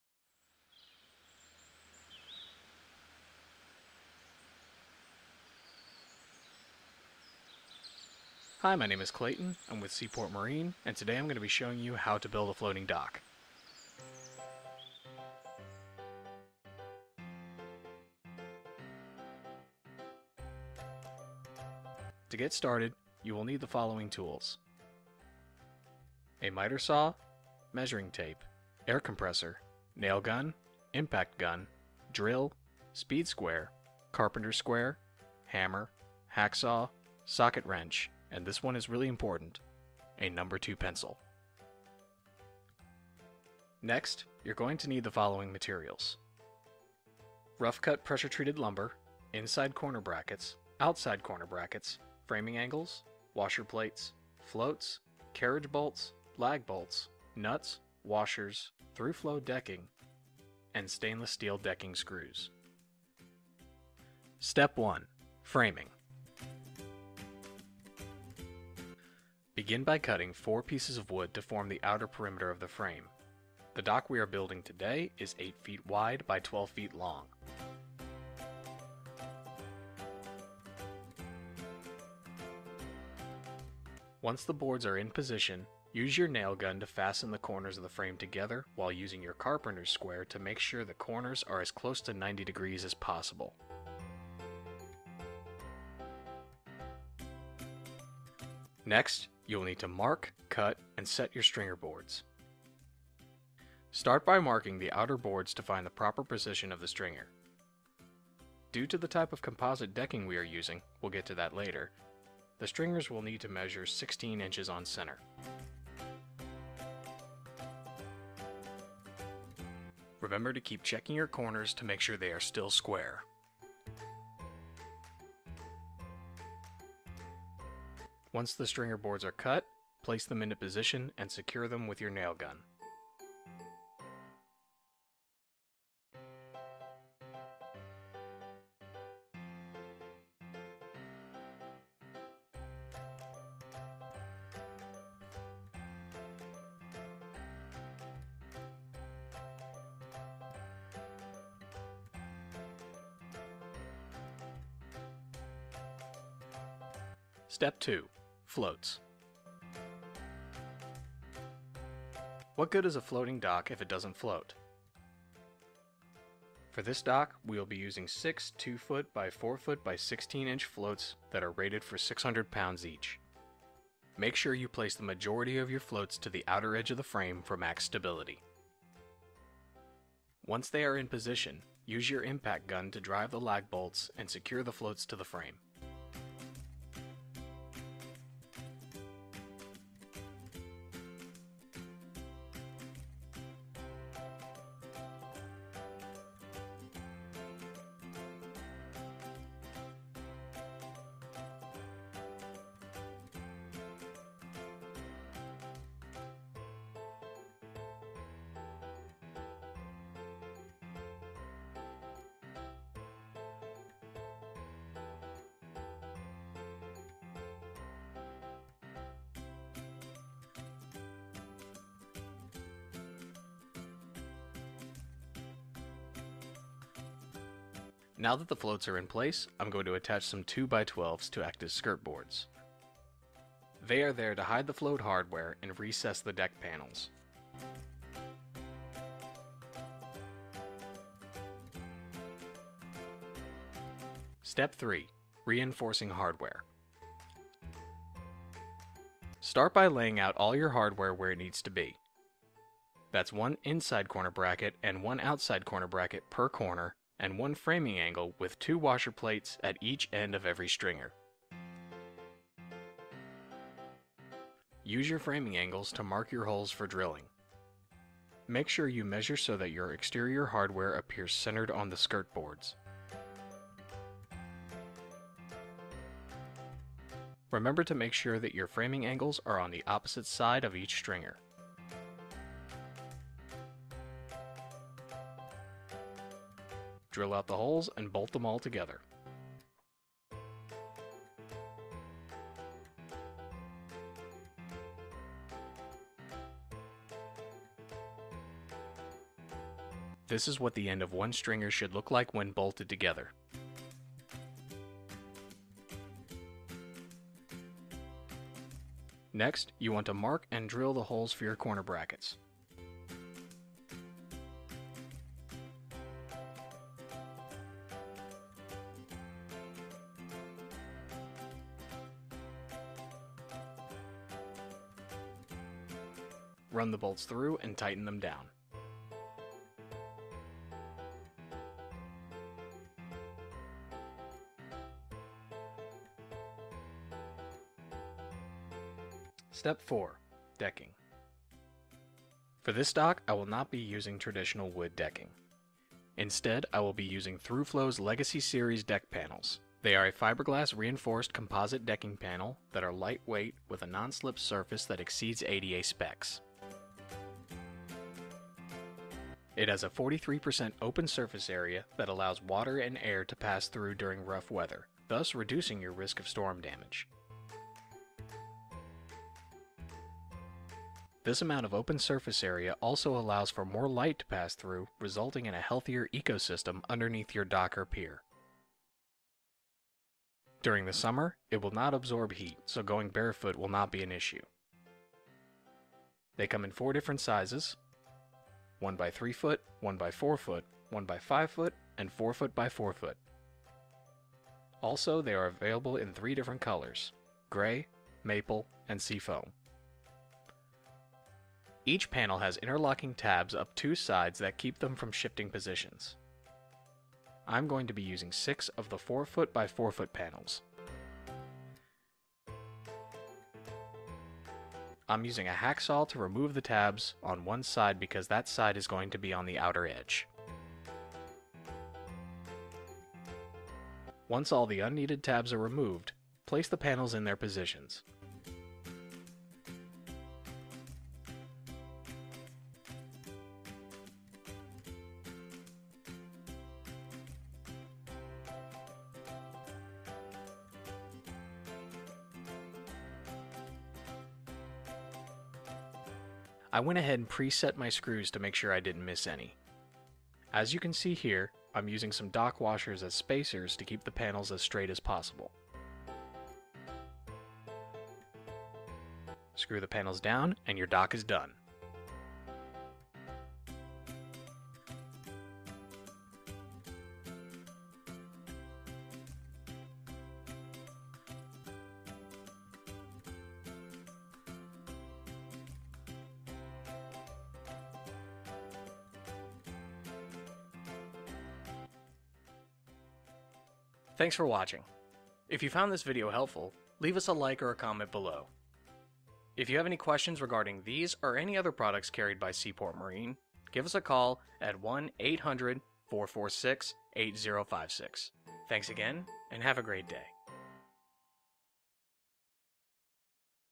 How to Build a Floating Dock - Sea Port Marine - Thruflow
In this video we will be discussing the basics of building a floating dock.

Sea Port Marine
812 professional place